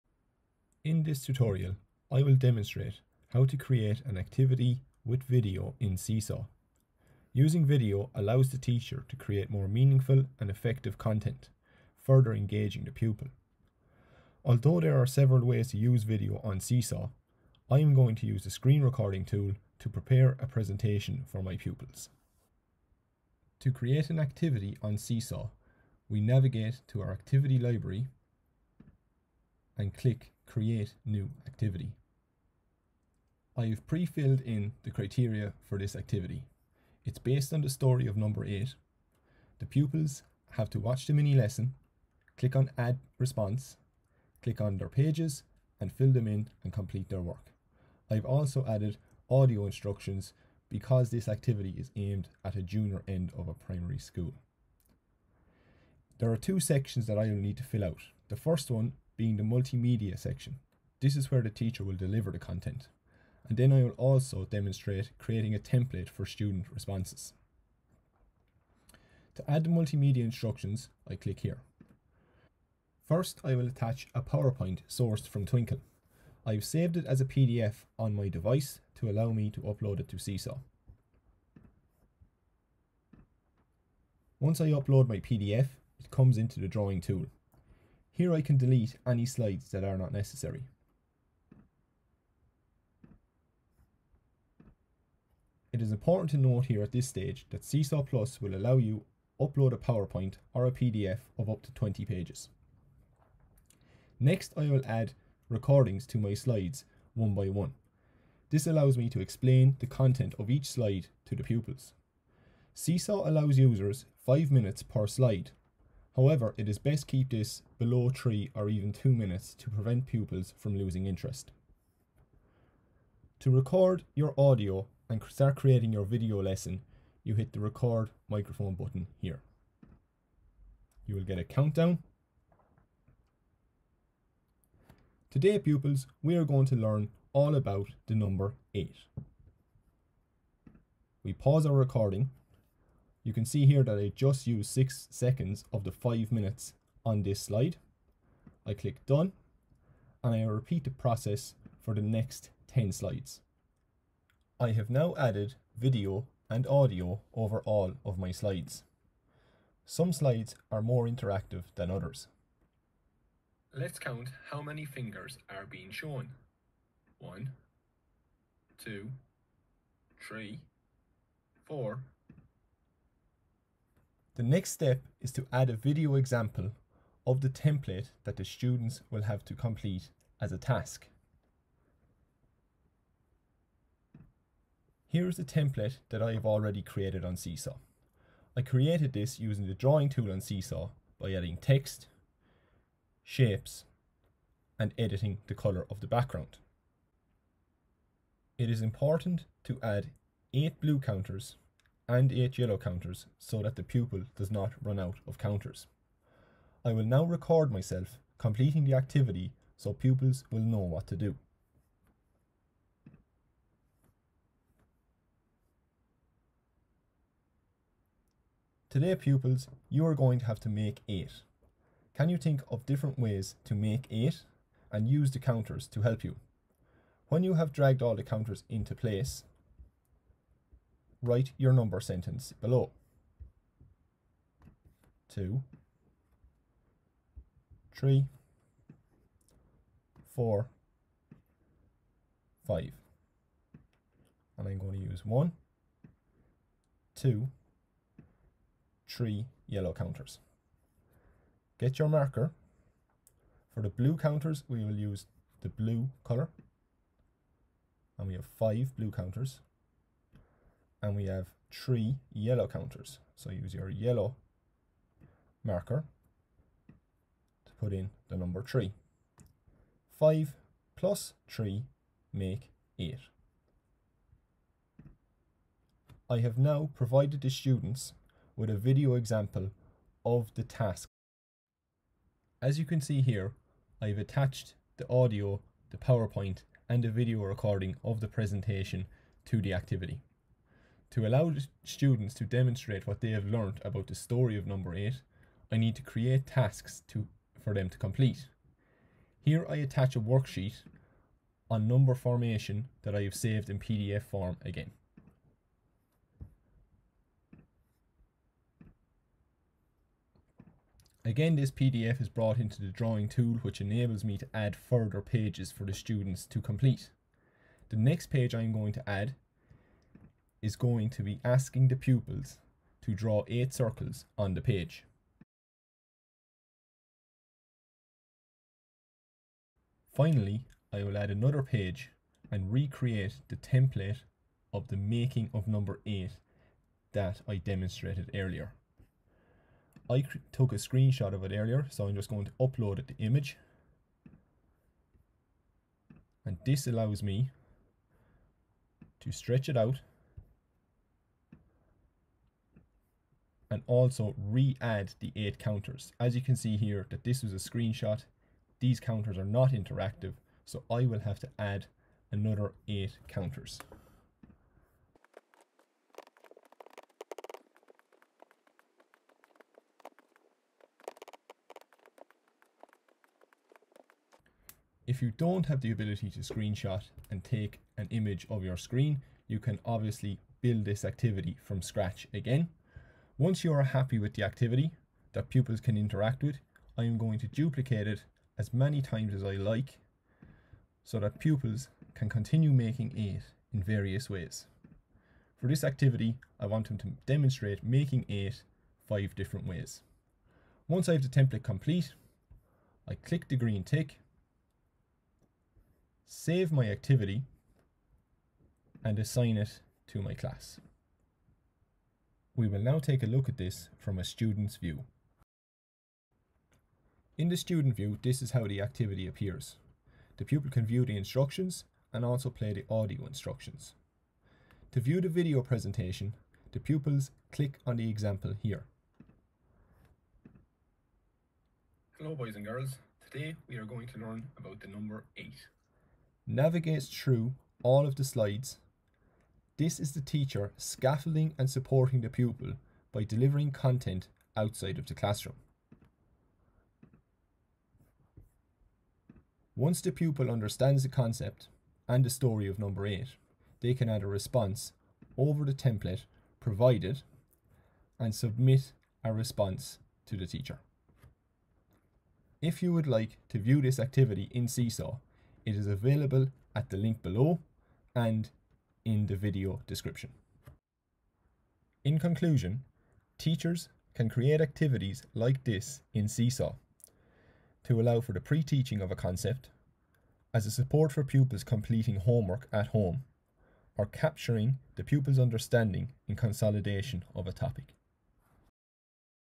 Seesaw - Creating a Video Activity
Using the screen recording tool in Seesaw allows teachers record and annotate powerpoint slides/PDFs on a topic or concept. This further engages the pupils with the learning process. This activity could be used to pre-teach content to pupils , as a support for pupils while completing homework or to consolidate and assess a pupils understanding of a topic.

Slides are owned by Twinkl.